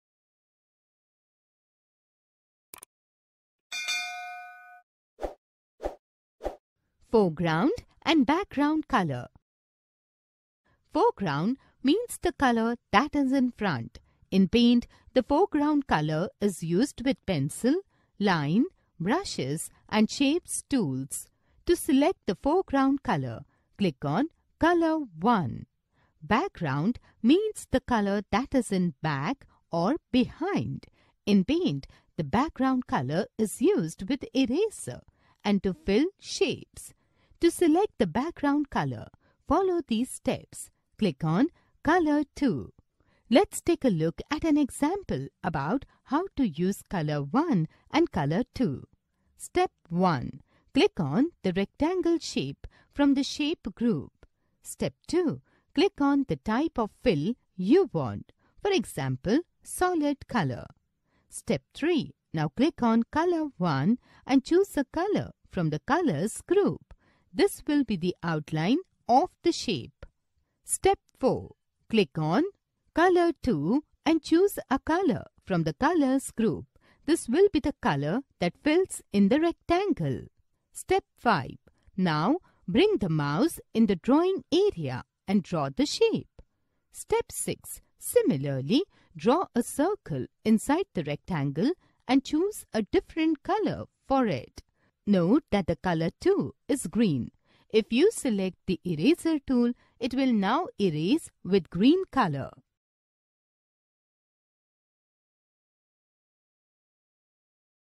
Foreground and Background colour
In this video, the basics of foreground and background colour will be introduced to the students.